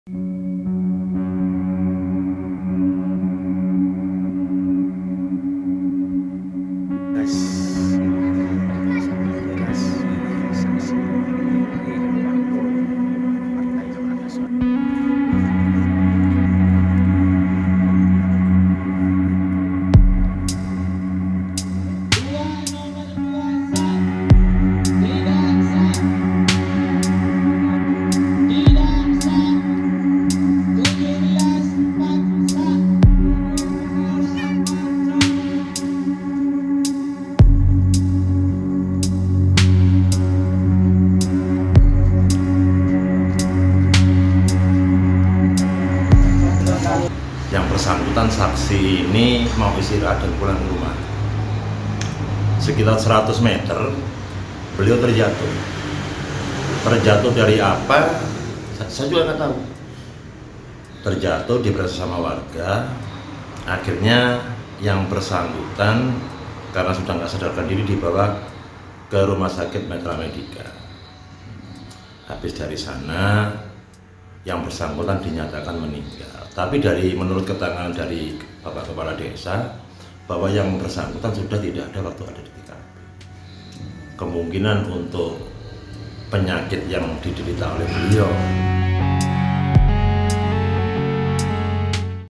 Saksi Pemilu 2024 di Bondowoso Meninggal Dunia Saat Bertugas di TPS, Diduga Korban Sakit & Kelelahan
Bondowoso, Kompas.TV Jawa Timur - Inilah Suasana Rumah Duka Seorang Saksi Partai Amanat Nasional Atas Nama Samson, 55 Tahun, warga Desa Bataan Kecamatan Tenggaran, Bondowoso, Jawa timur, kamis dini hari.

Korban diketahui sejumlah warga, terjatuh dalam perjalanan pulang dan tak sadarkan diri setelah beristirahat menjadi saksi di tempat pemungutan suara 24 di desanya.

Korban langsung dibawa ke Rumah sakit Mitra Medika dan setelah dilakukan pemeriksaan medis dinyatakan meninggal dunia.

Hasil pemeriksaan Polisi, korban diduga kesehatannya terganggu dan tetap memaksakan diri hingga mengalami kelelahan setelah seharian menjadi saksi.

Atas permintaan keluarga, korban yang diketahui sebagai warga yang supel dan pekerja keras tidak menjalani proses otopsi, kematiaanya dianggap sebuah takdir.